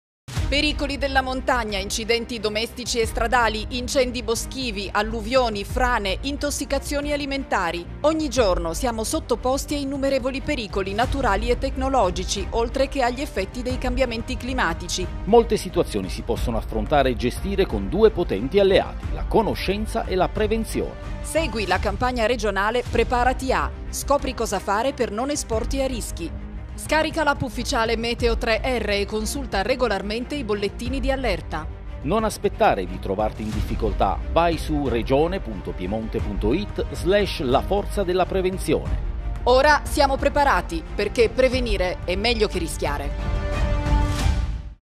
RISK COM - La forza della prevenzione
Campagna regionale "Preparati a: scopri cosa fare per non esporti ai rischi".